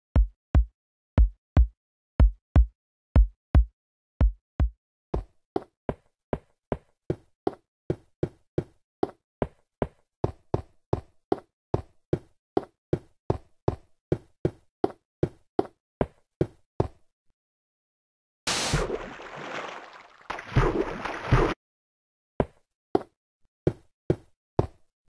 Backwards pumpkin
Well I guess notch screwed up and we have backwards pumpkins, Way to go notch